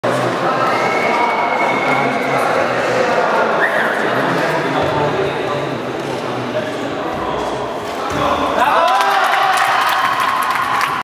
Kostela Piotr kontra Wojciech Batkiewicz
Rzut karny w meczu KSWorwa Szarotka Nowy Targ - KS Madex Górale Nowy targ.